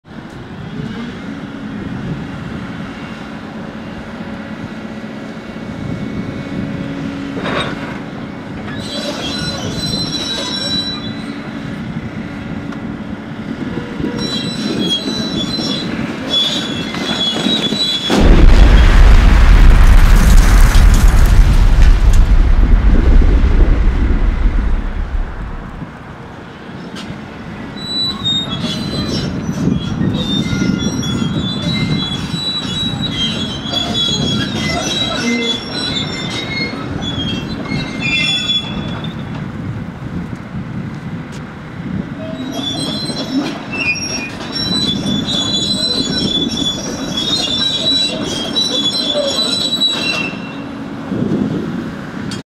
Valley Fair Mall Demolition
Construction crews are making way for the new Larry H. Miller Megaplex movie theaters at Valley Fair Mall. The final wall of the old Mervyn's building came down with a massive rumble.